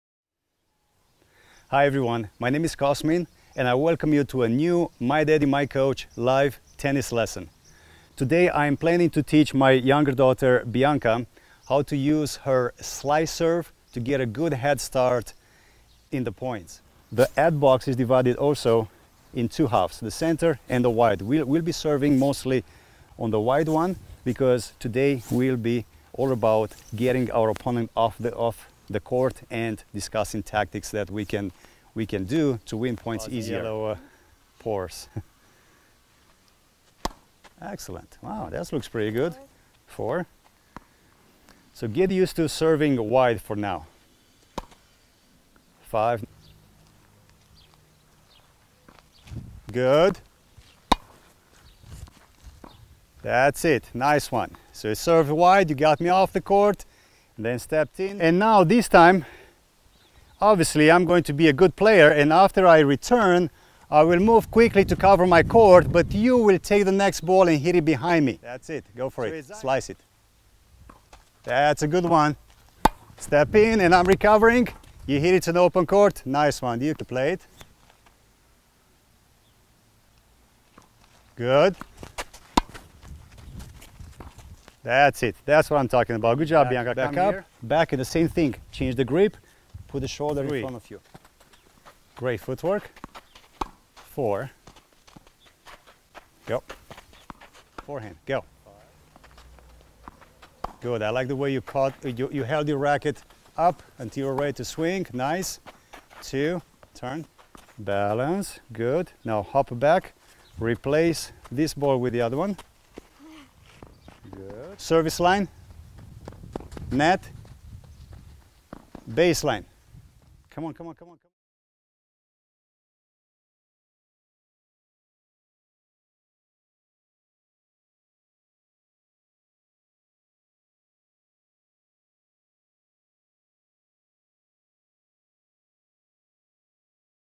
My Daddy / My Coach - Kids Tennis Lesson # 26 (Intro)
A preview of the new My Daddy / My Coach live tennis lesson #26 where a coach/father teaches his daughter how to use her slice serve to win points in tennis more easily.